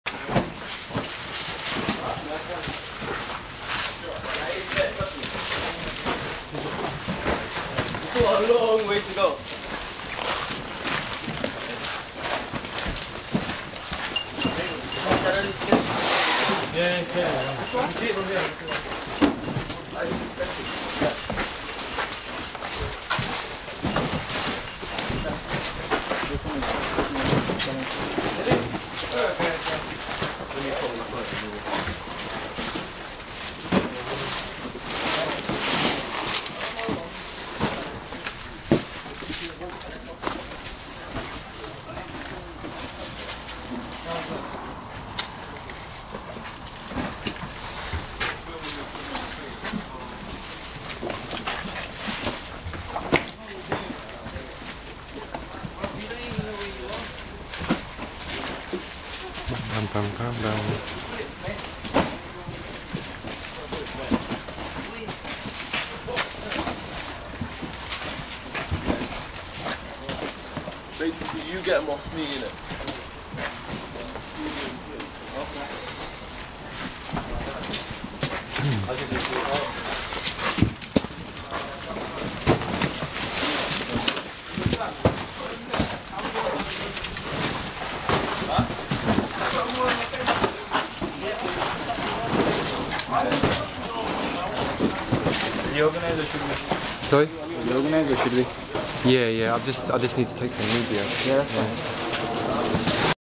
Premier Palletised Limited donates it's services for Pakistan Flood Relief 2010
Premier Palletised Limited donates it's services for Pakistan Flood Relief. The drivers and volunteers all helped to upload the 1,500 cartons of food and essentials onto the 44ft Artic that was heading for Heathrow Airport.